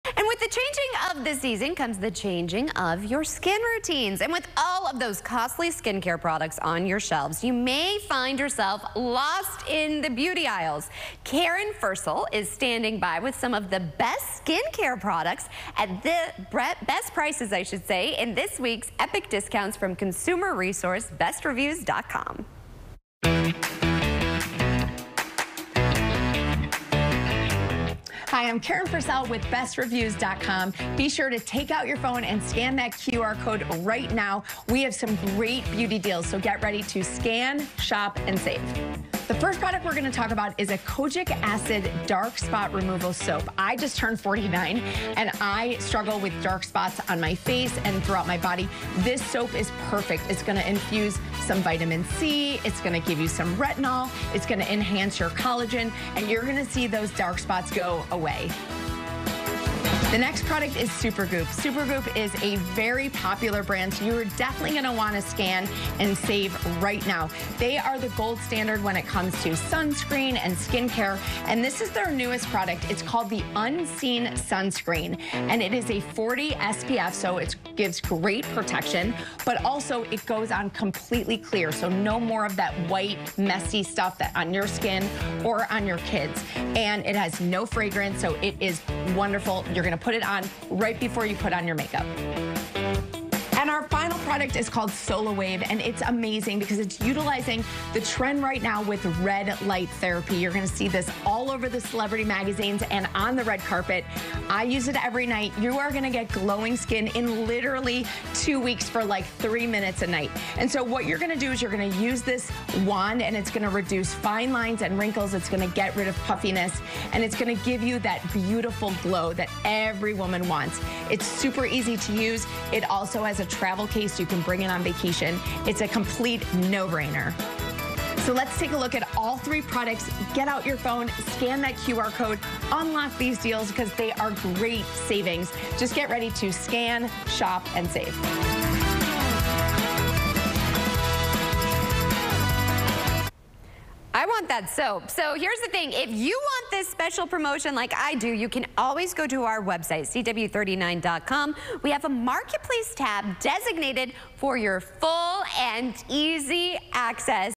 Epic Discounts by BestReviews.com | Houston Happens
With the changing of the season comes the changing of your skin routines. And with all of the costly skin care products on shelves, you may find yourself lost in the beauty aisles. Karen Firsel is standing by with some of the best skin care products at the best prices in this week's Epic Discounts from consumer resource BestReviews.com.